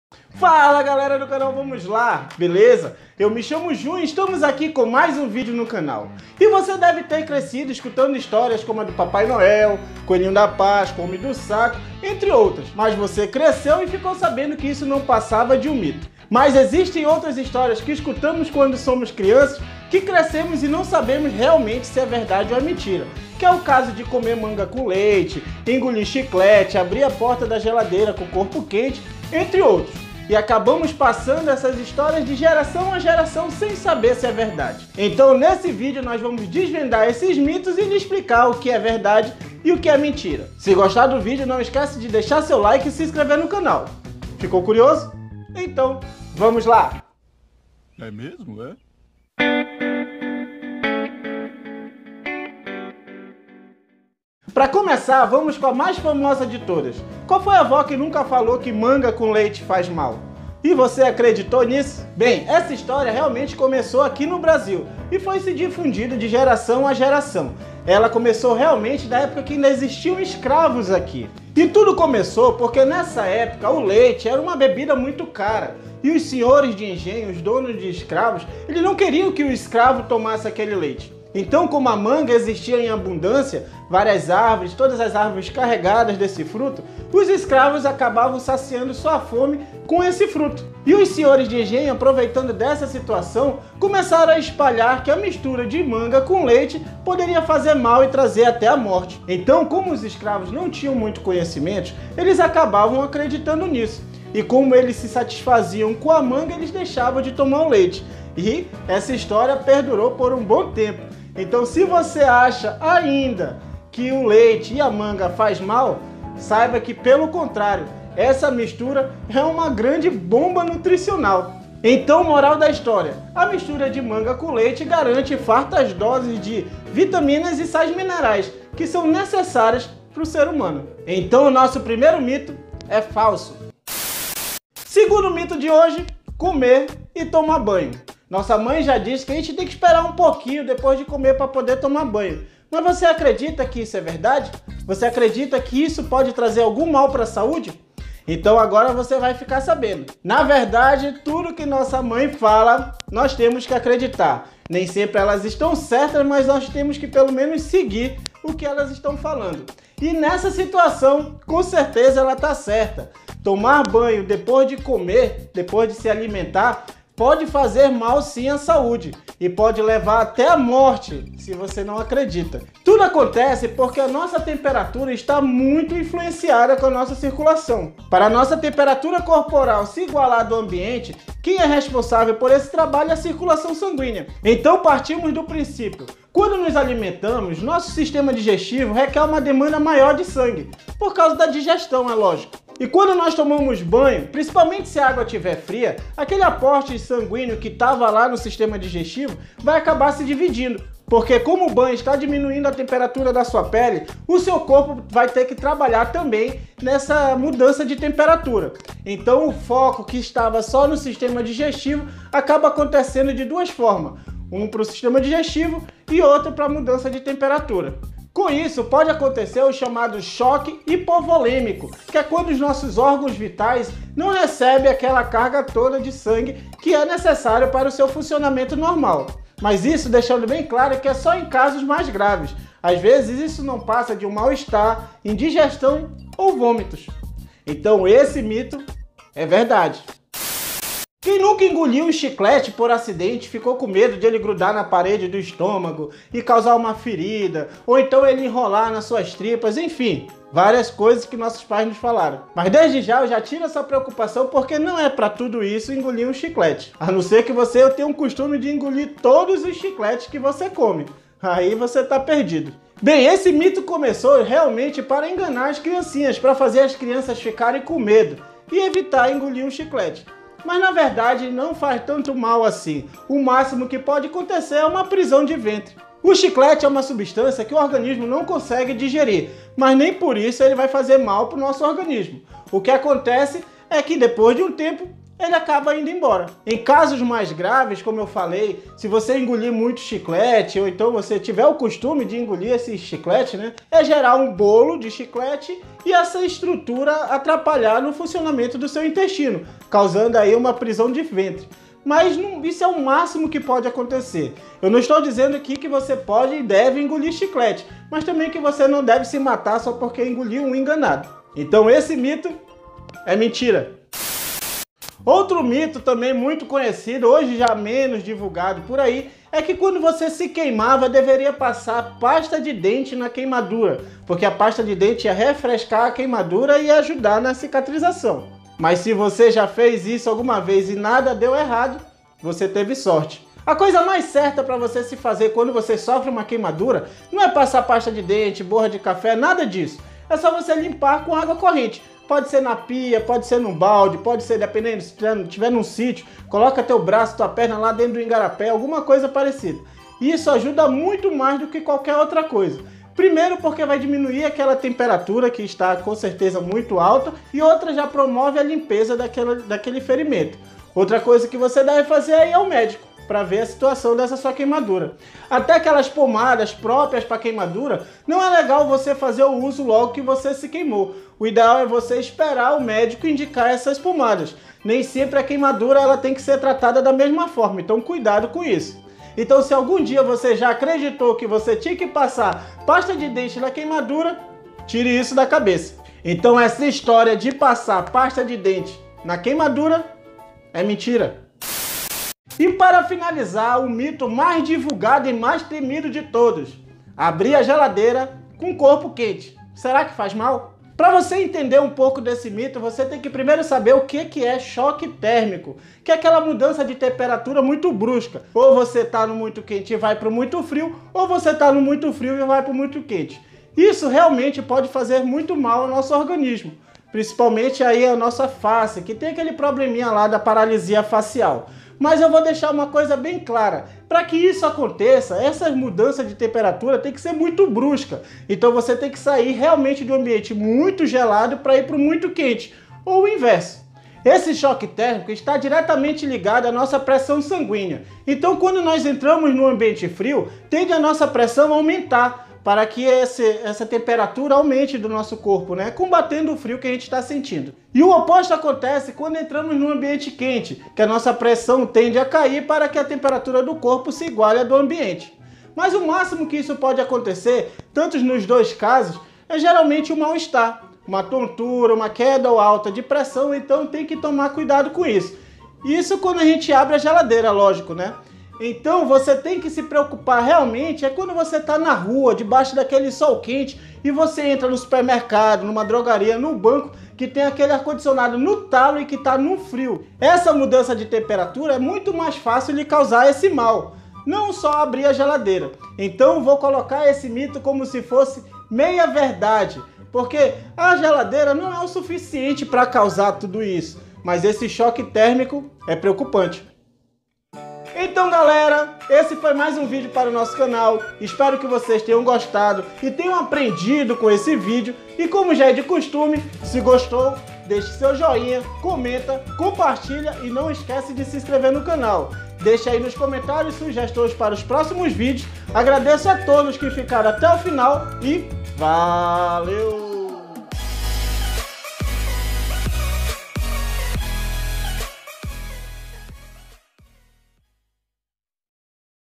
TOP 5 | MITOS DA INFÂNCIA
Fala falera do canal vamos lá! Blz?!
Nesse vídeo trago para vocês o TOP5 | MITOS DA INFÂNCIA
Desde a manga com leite até engolir chiclete ou abrir a geladeira com o corpo quente.

Valeu